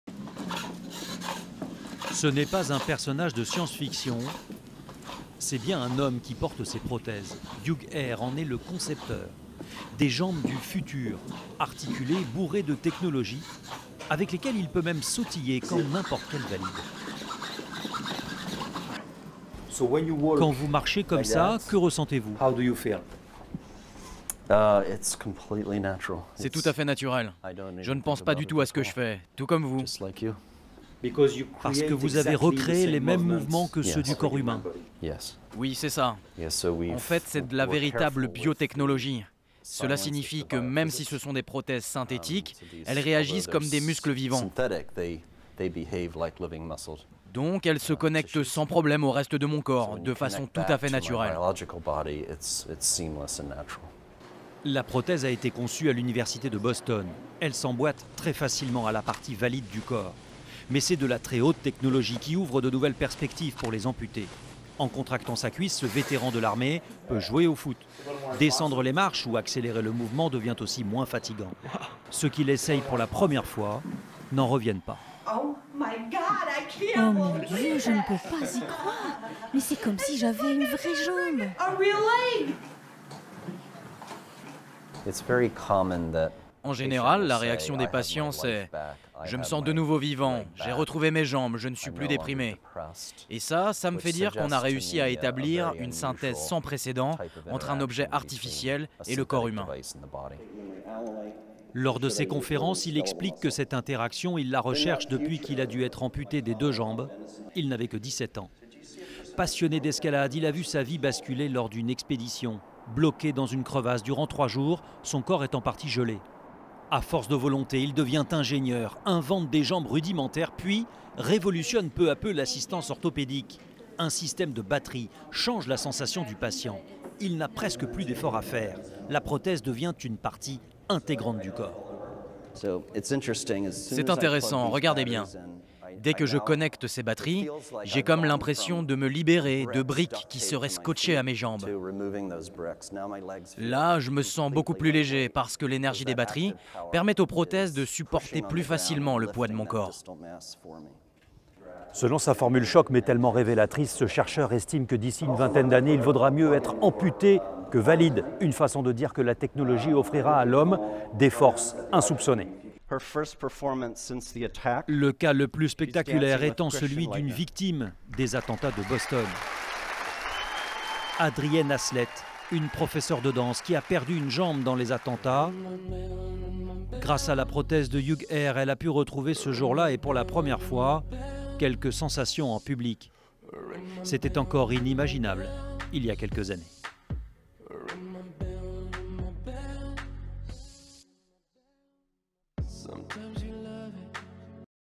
Les incroyables prothèses de Hugh Herr
Reportage de Jacques Cardoze, Régis Massini, Arielle Monange et Sherron Lumley.